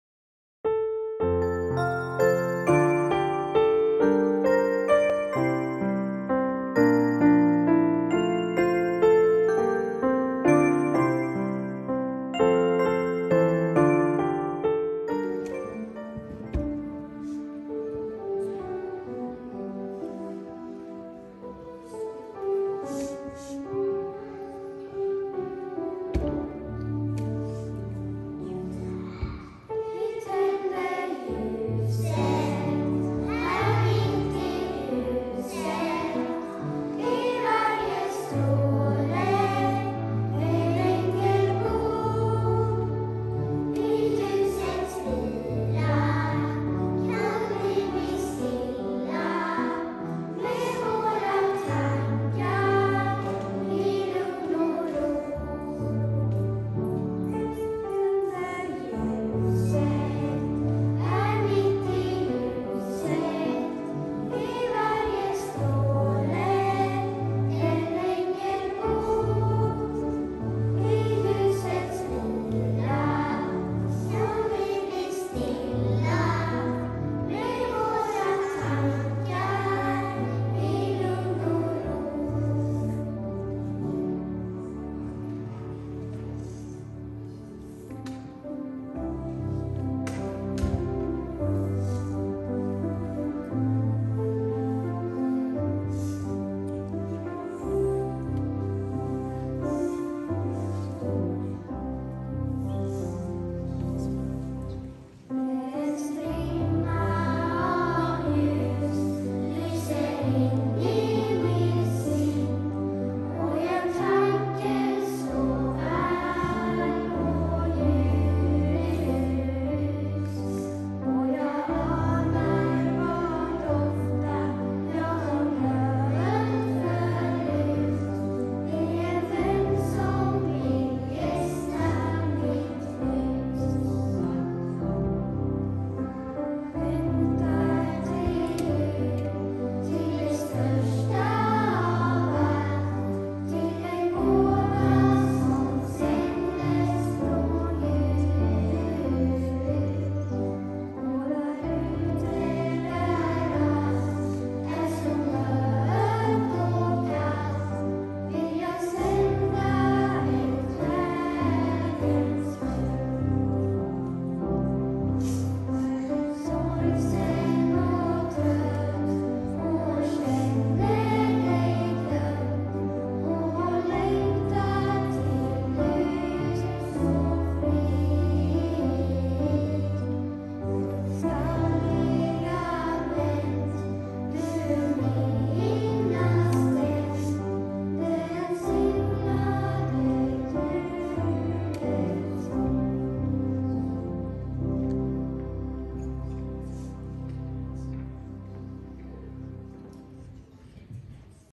Julkalender 2021- 16 december
I dagens lucka möter ni barnen som sjunger i